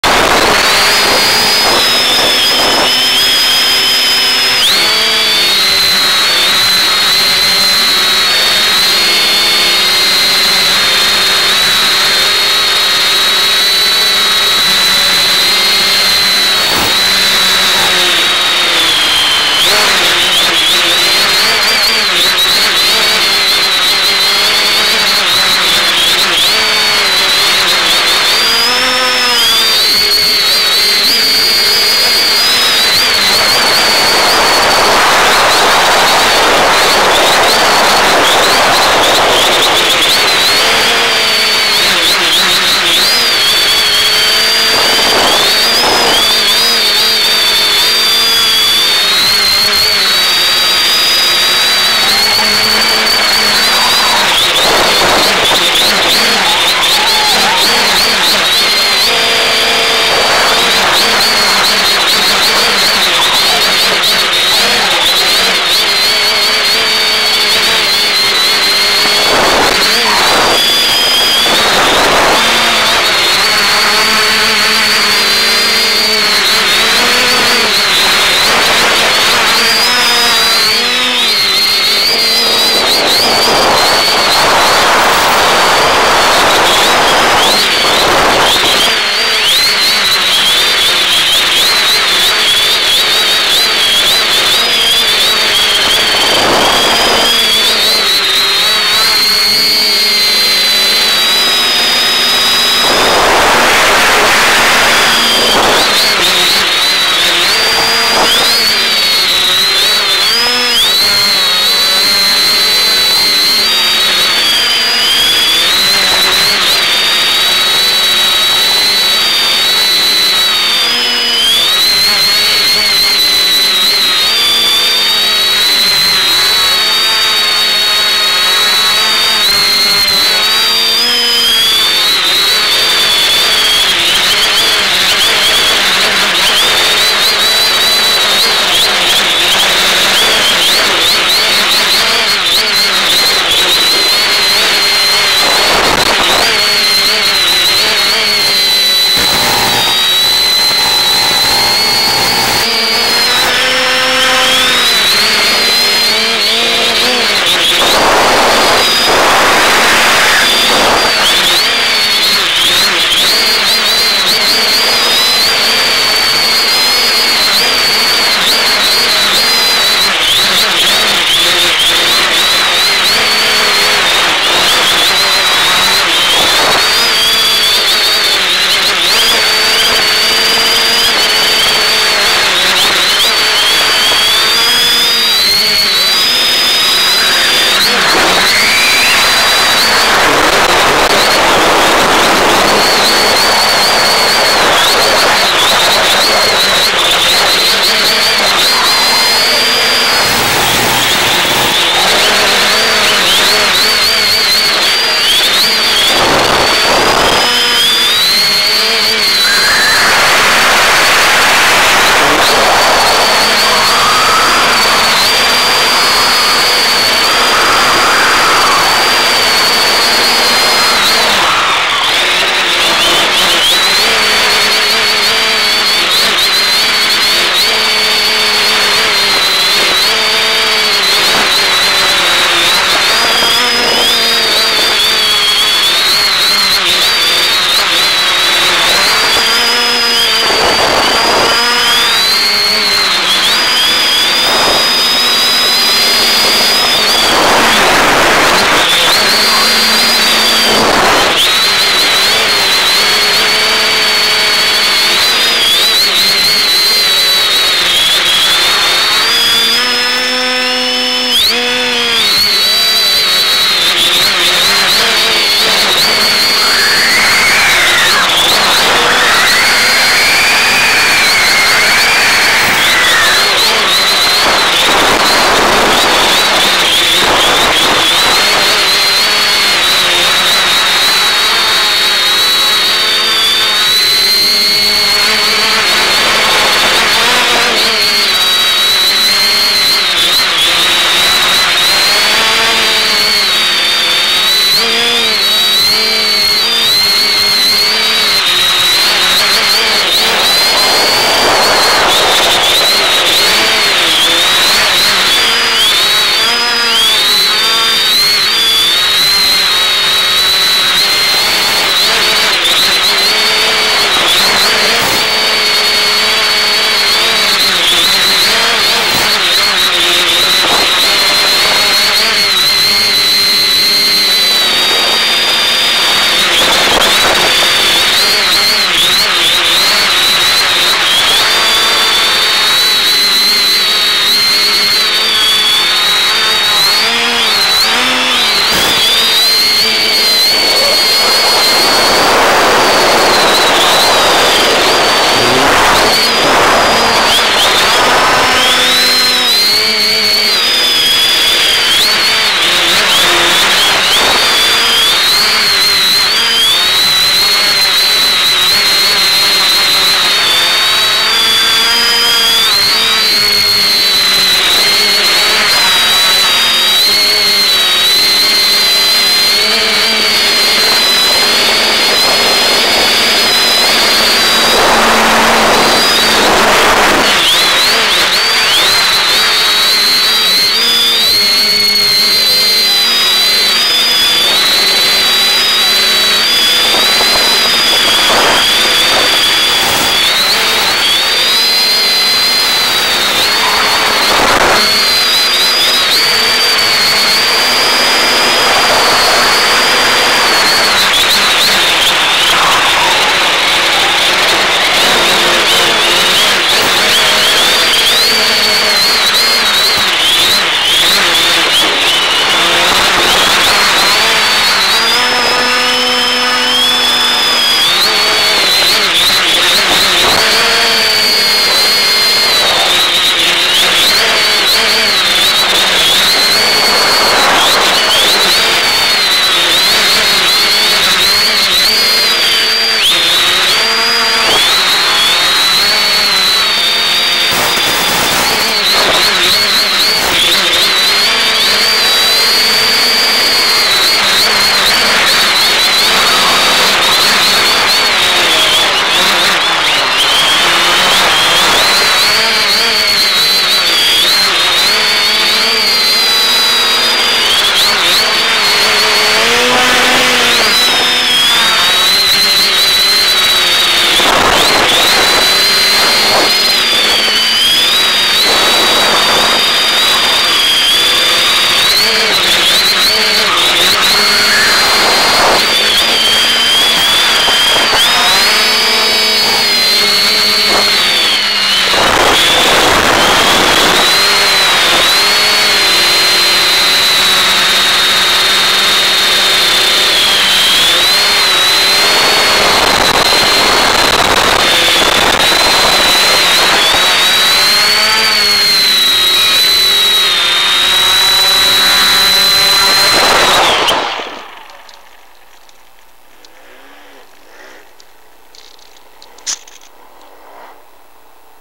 X550AIOP FPV DL Holden 2013-06-21
Recording of downlink video while flying my X550AIOP quadcopter via FPV, QMFC Holden Field, 6/21/2013.  See here for the higher-quality keycam video